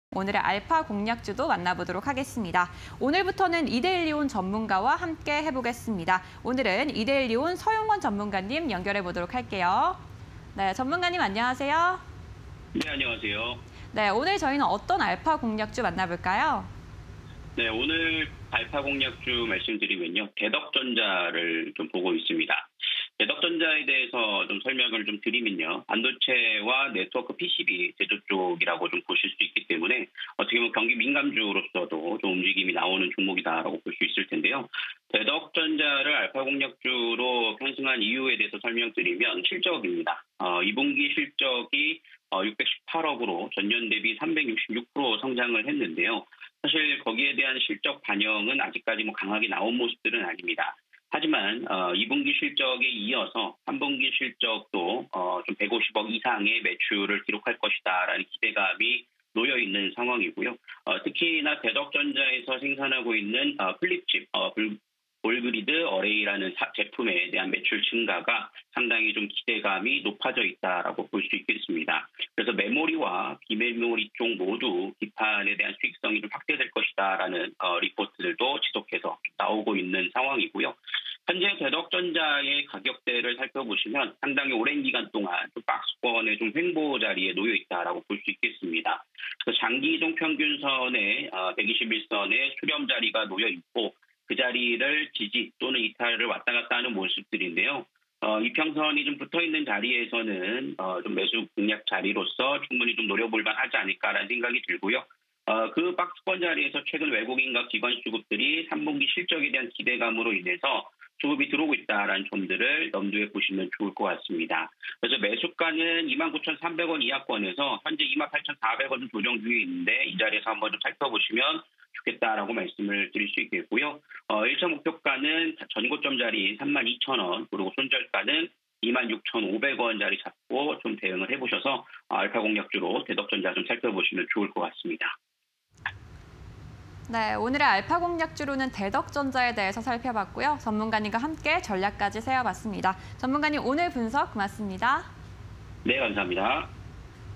대덕전자(353200)... 2분기 영업익 618억원… 전년대비 +366% 상승_알파 공략주 (20220905)
- 반도체 및 네트워크 PCB 제조
- 2분기 영업익 618억원… 전년대비 +366% 상승
- 메모리·비메모리 기판 수익성 확대
- 기술적 수렴 후 상승세… 수급 개선
- 120일선 수렴자리 이용한 매수 가능